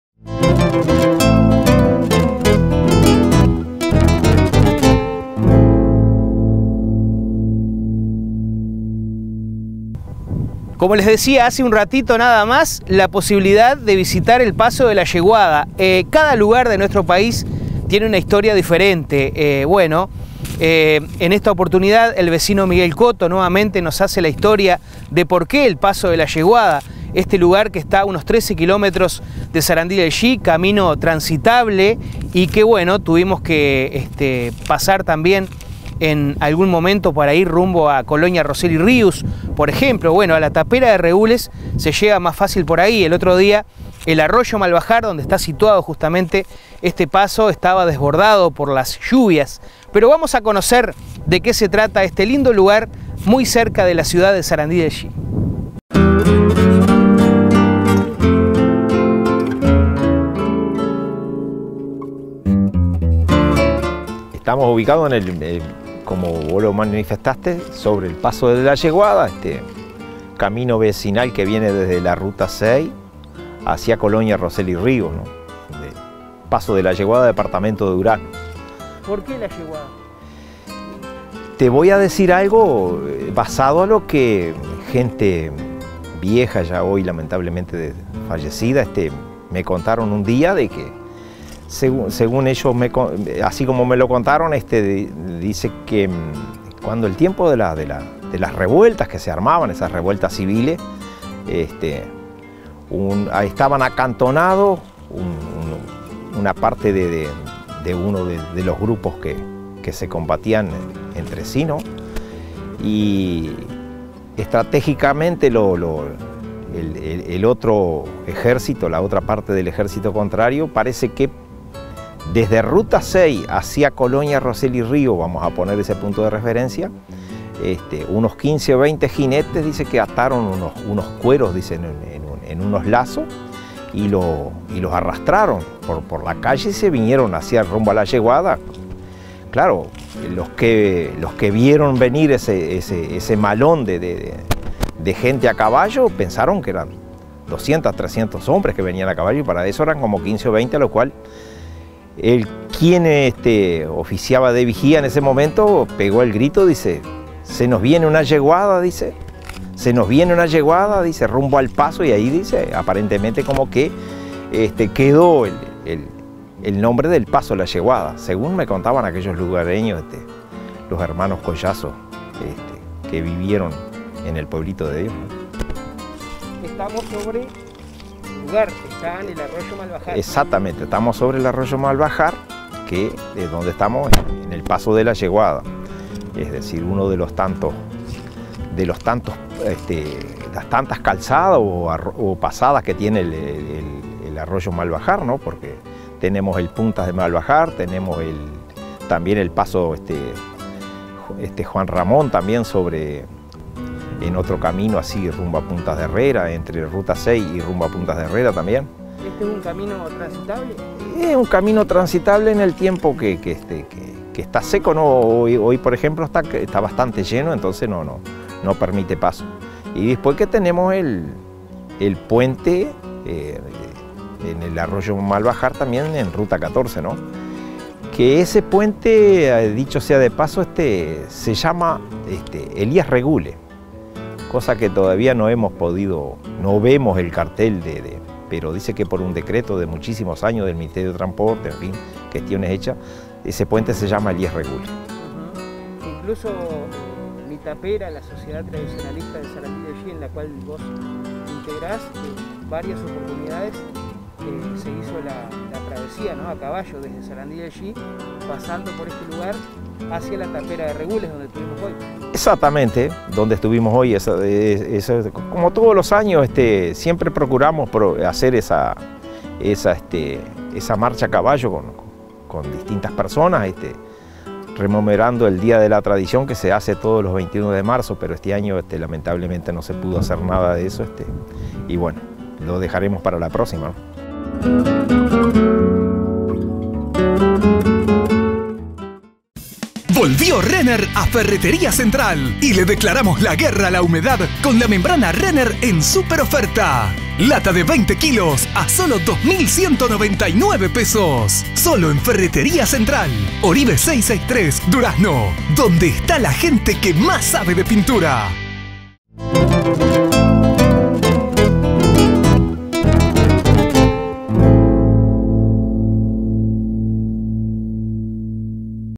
LLEGAMOS HASTA EL "PASO DE LA YEGUADA" EL LUGAR ESTA UBICADO SOBRE EL ARROYO MALBAJAR
DESPUÉS DE HABER VISITADO "LA TAPERA" DEL DR. ELÍAS REGULES, CONTINUAMOS EL RECORRIDO POR CAMINO VECINAL, LLEGAMOS HASTA EL "PASO DE LA YEGUADA" EL LUGAR ESTA UBICADO SOBRE EL ARROYO MALBAJAR A UNOS 12 KILÓMETROS DE LA CIUDAD DE SARANDI DEL YI